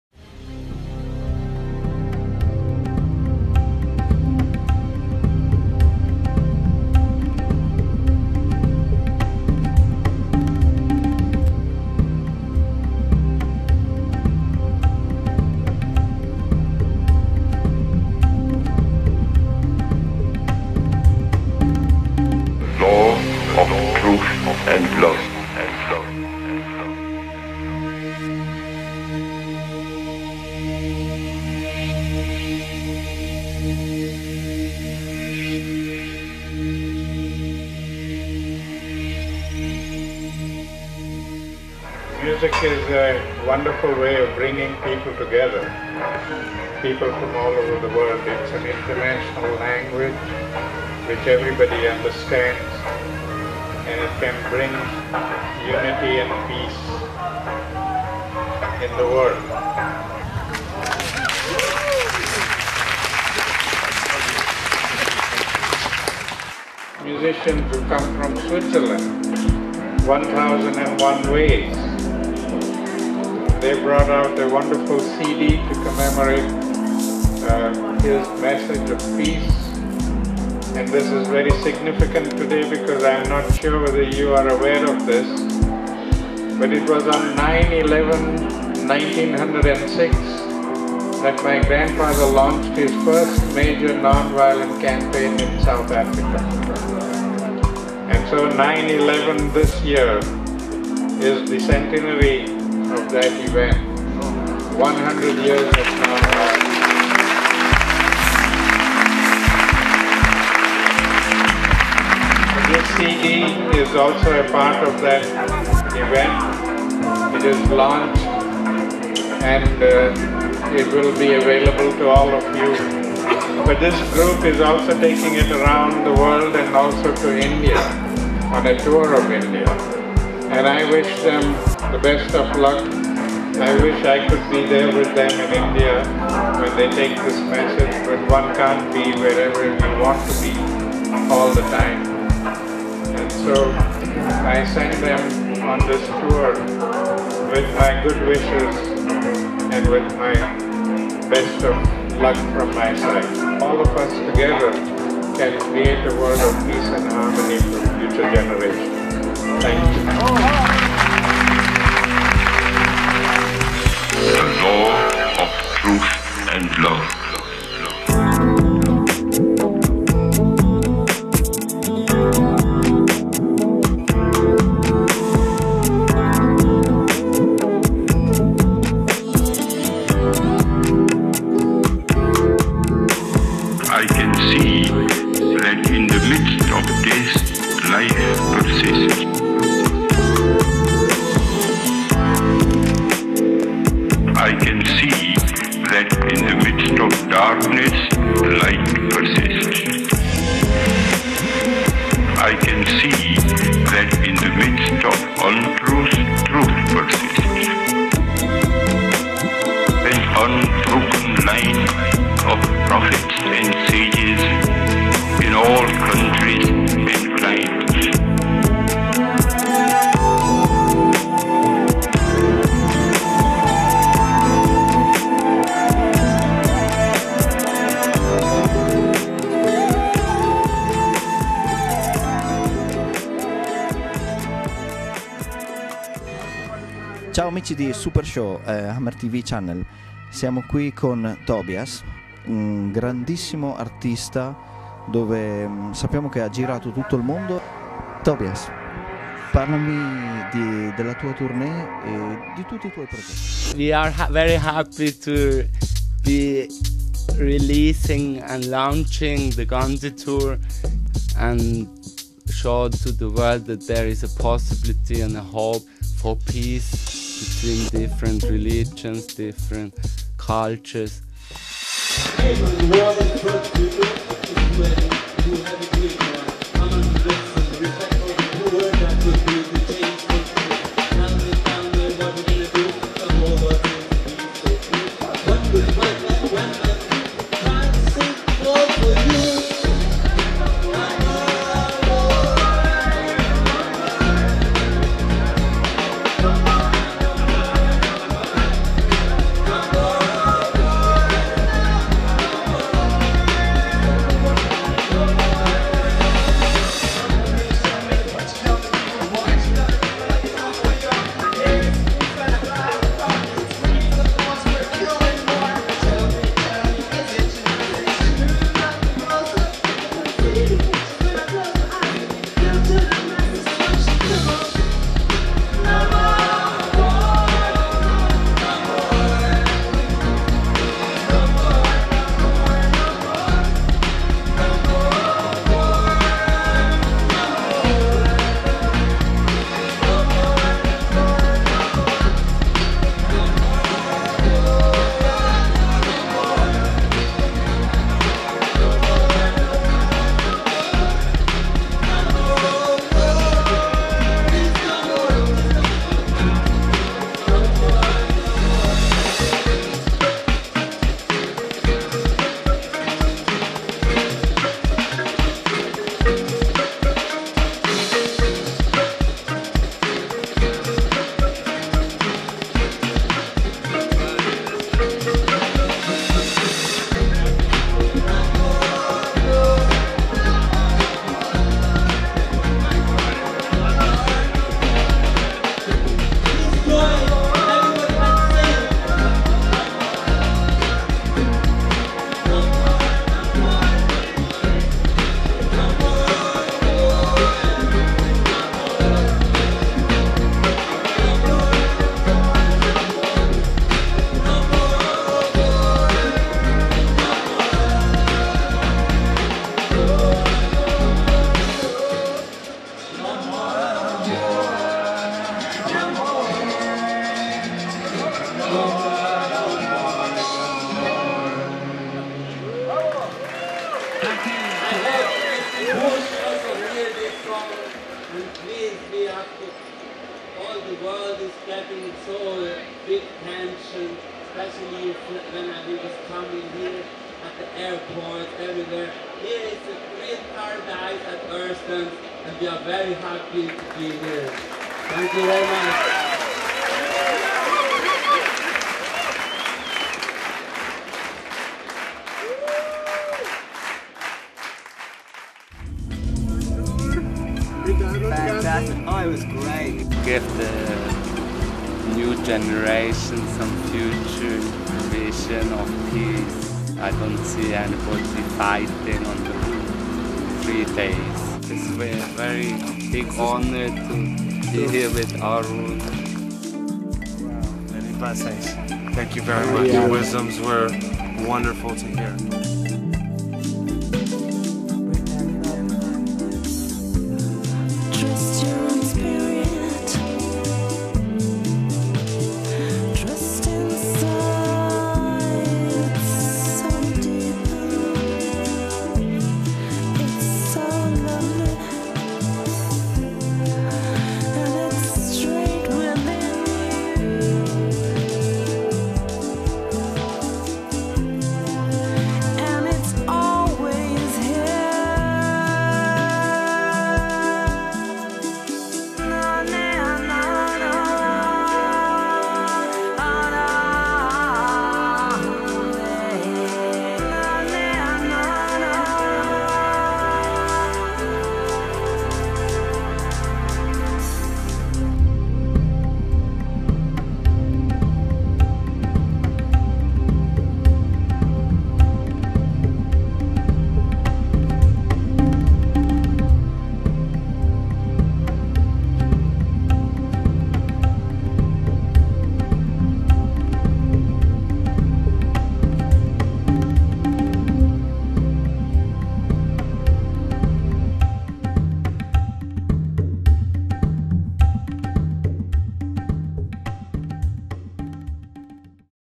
Music 4 Peace / Gandhi-Tour @ Earthdance / 100 Years Gandhi's "Satyagraha"
Celebrate 100th Anniversary of Gandhi's Nonviolence Action "Satyagraha"
The Launch of the Gandhi-Tour
at the 10th EARTHDANCE PEACE FESTIVAL hub event in California
on September 15th 2006
with the 1001 WAYS (world beat) multimedia live act
and special guest Arun Gandhi.

Music For Peace
Speaker: Arun Gandhi
Orig. recorded Voice: Mahatma Gandhi